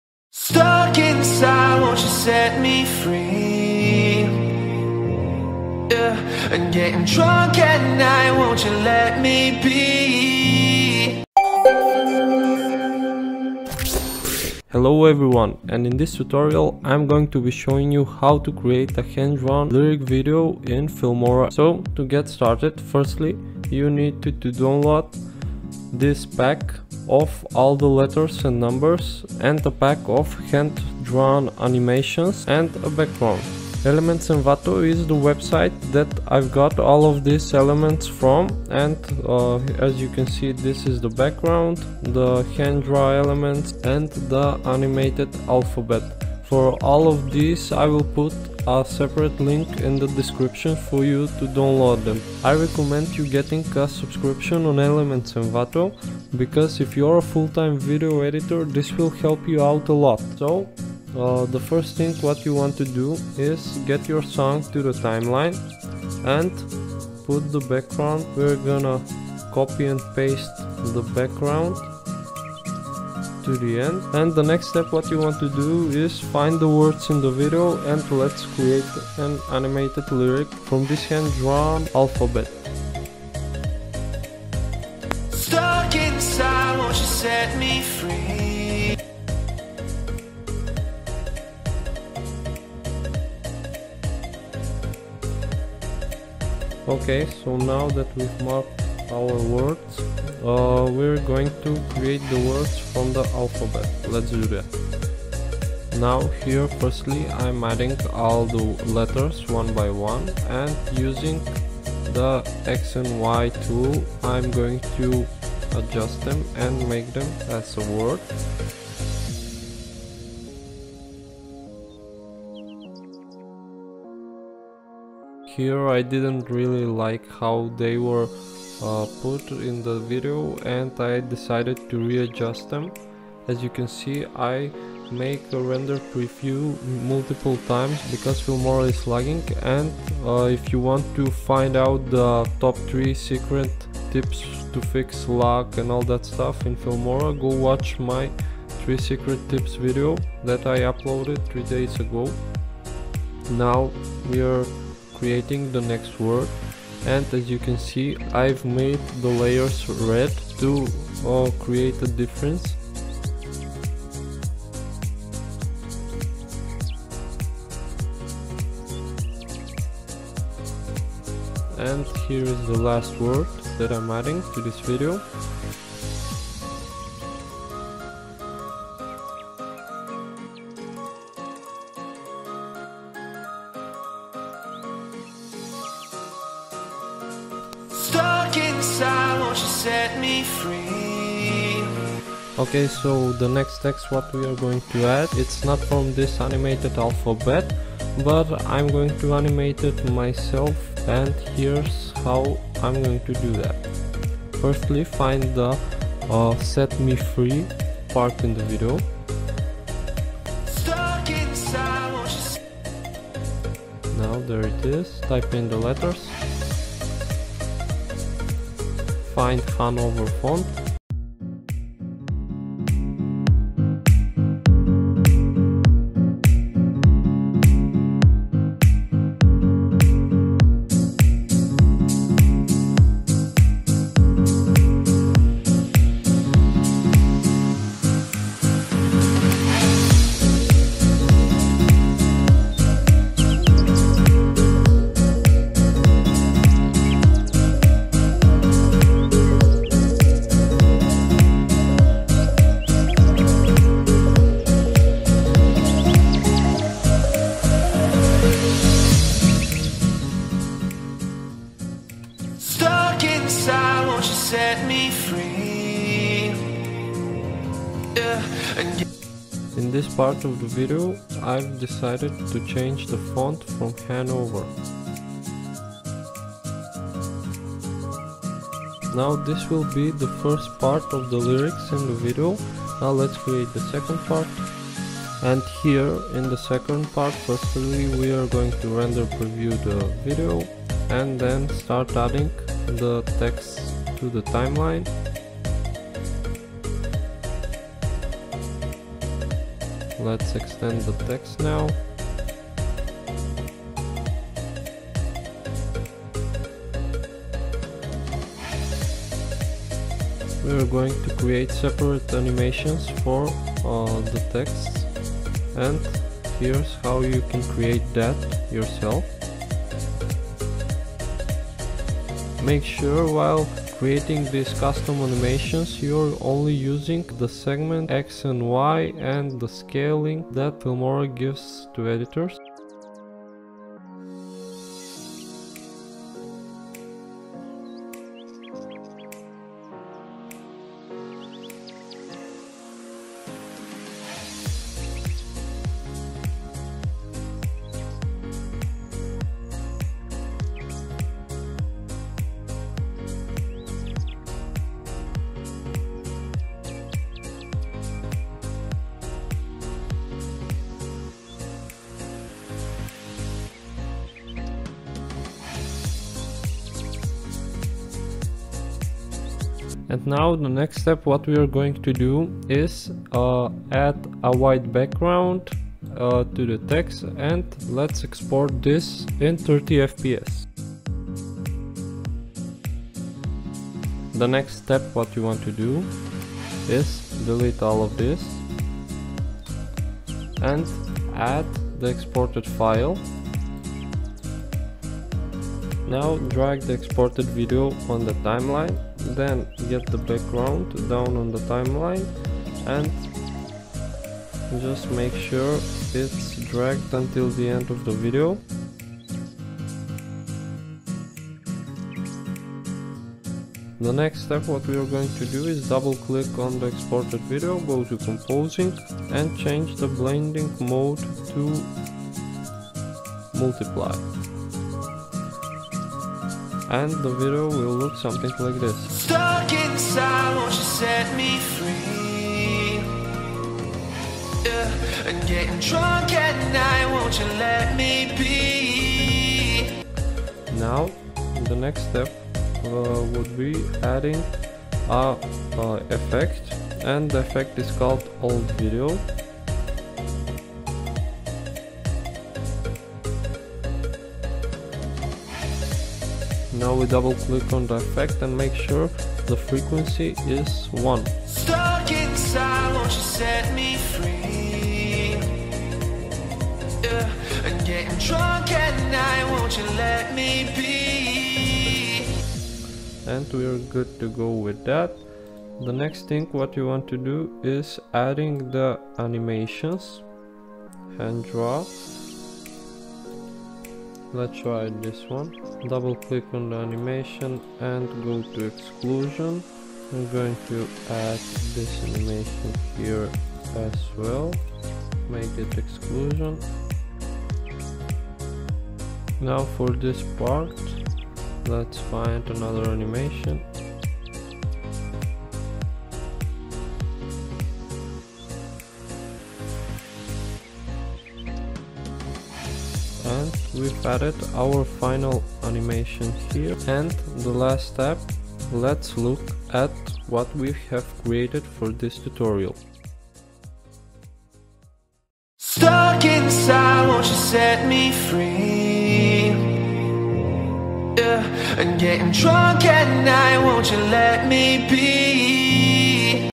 How to Make a Hand Drawn Animated Lyrics Video | Filmora X Tutorial
Hey guys, This is David and In this video, I will show you how to create a cool hand drawn lyrics video in Wondershare Filmora 10.1.